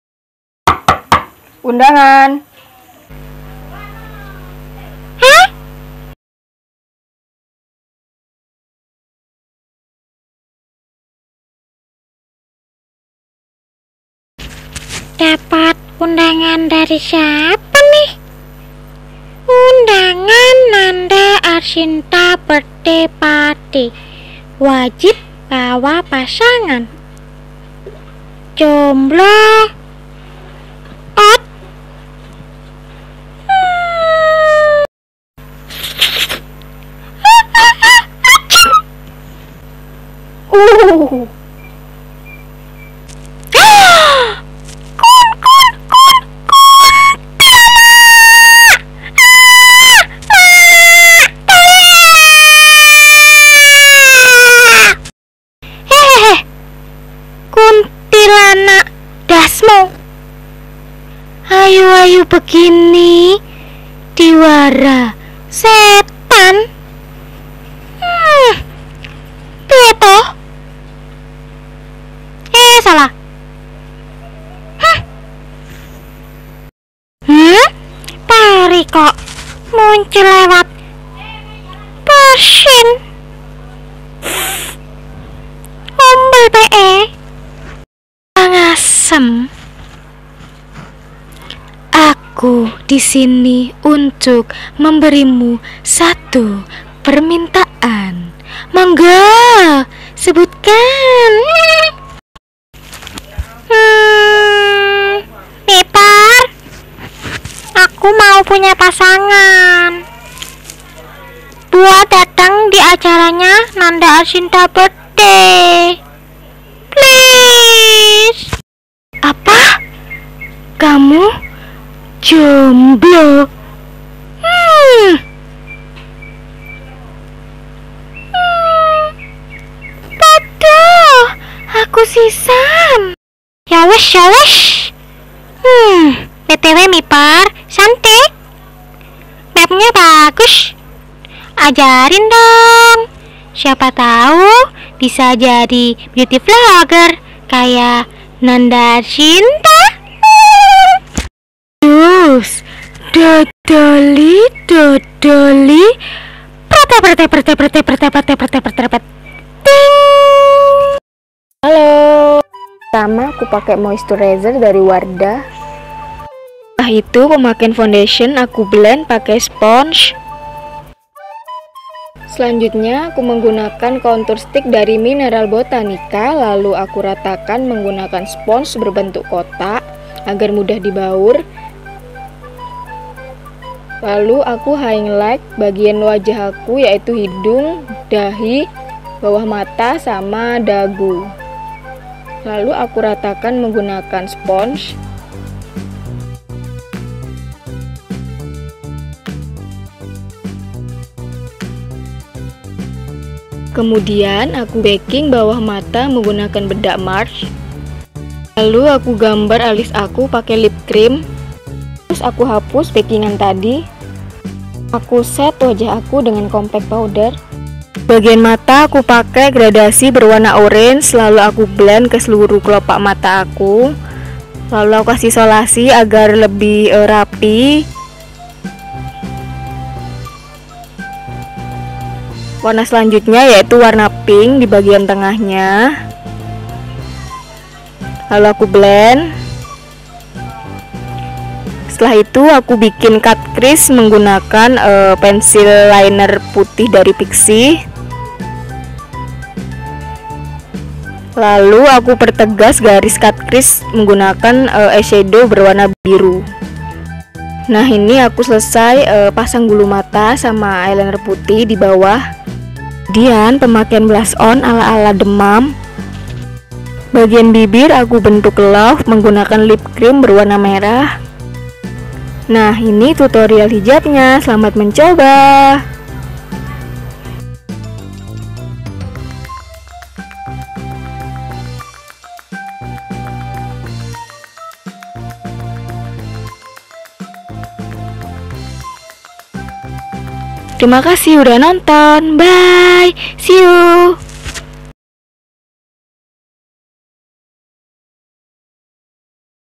MIMI PERI MAKE UP FANTASY NANDAARSYINTABDAY | SIMPLE CUT CREASE MAKE UP TUTORIAL | MAYA EMS
Assalamualaikum shalihah 💕💕

Di videoku kali ini aku lagi berpartisipasi dalam acara

Btw happy birthday untuk kak nanda arsyinta semoga semakin sukses , dilimpahkan rejeki, kesehatan dan umur yg panjang amin ya rabb 🙏

Semoga kalian terhibur yaa hehehe 🙈

Silahkan beri masukan positif dan request video untuk selanjutnya insya allah bakal aku balas ❣

Produk-produk yg digunakan :
1. Wardah Aloe Hydramild Multifunction Gel (Rp. 30.000)
2. La Tulipe Stay Smooth Liquid Foundation Shade 02 (Rp. 60.000)
3. Mineral Botanica Highlight And Contour Stick Shade C. Cookies (Rp. 59.000)
4. Marcks Bedak Beauty Powder 40 gr Shade Creme (Rp. 15.000)
5. Mukka Lip Cream Matte (Rp. 15.000)
4. Emina Bare With Me Mineral Compact Powder Shade Amber (Rp. 35.000)
5. Implora Eyeshadow (Rp. 25.000)
6. Pixy Line and Shadow Shade White (Rp. 39.000)
7. Viva Fin Touch Shade 03 Red Pink (Rp. 9.000)
8. Bilqis Eyelashes

Selamat mencoba girls 💖💖

Mohon maaf jika ada salah kata atau kekurangan lain 😇

Alat perekam : Handphone
Editing : Filmorago Android

Wassalamu'alaikum 💕💕